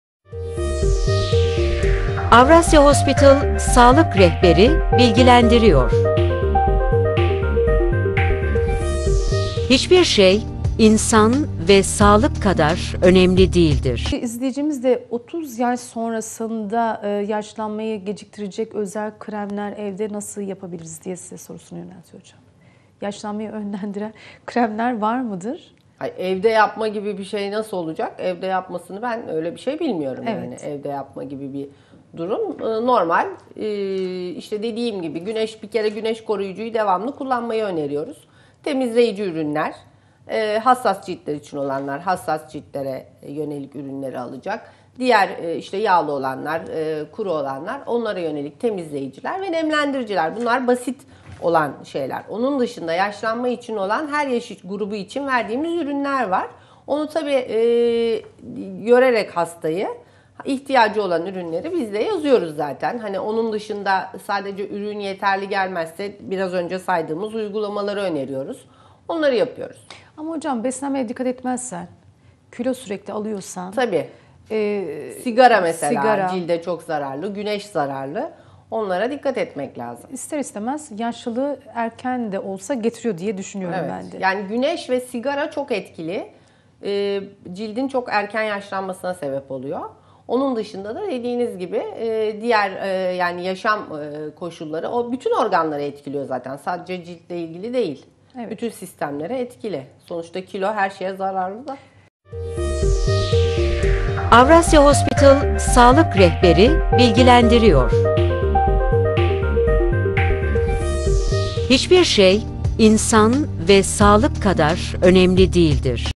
Yaşlanmayı Nasıl Geciktiririz - Cilt Hastalıkları Uzmanı Dr.Hatice Deniz YARDIMCI
Cilt Hastalıkları Uzmanı Dr.Hatice Deniz YARDIMCI - Yaşlanmayı Nasıl Geciktiririz